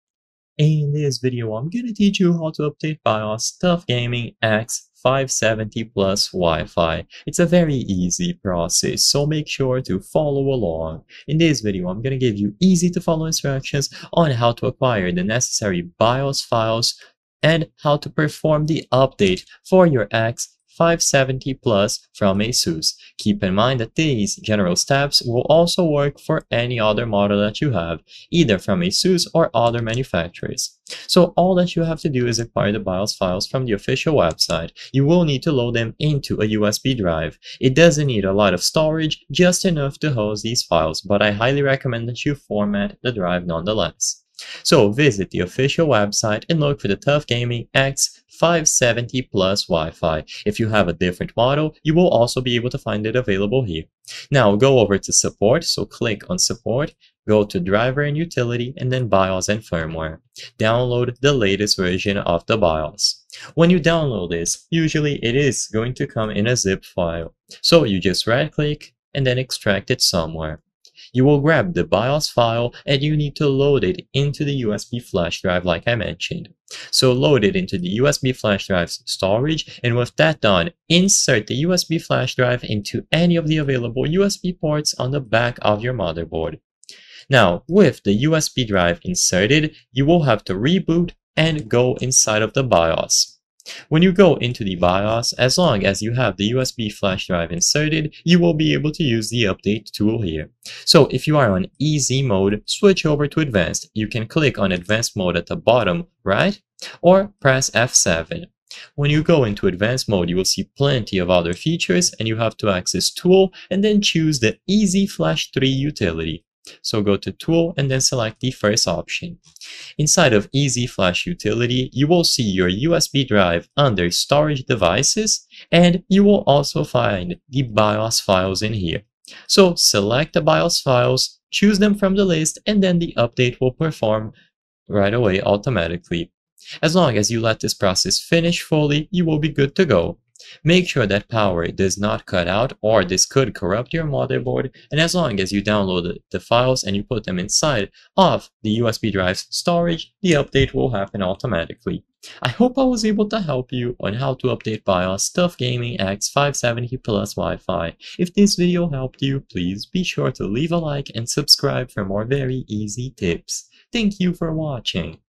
How To Update BIOS TUF GAMING X570-PLUS WIFI (2025) - Step by Step
I will solve your doubts about how to update bios tuf gaming x570 plus wifi, and whether or not it is possible to do this.

I hope I have helped you with the video!